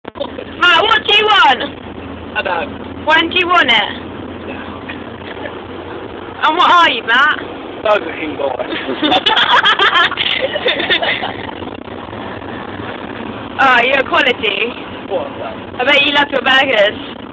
Matt Bird Burger boy
Matt on the coach home from thorpe park x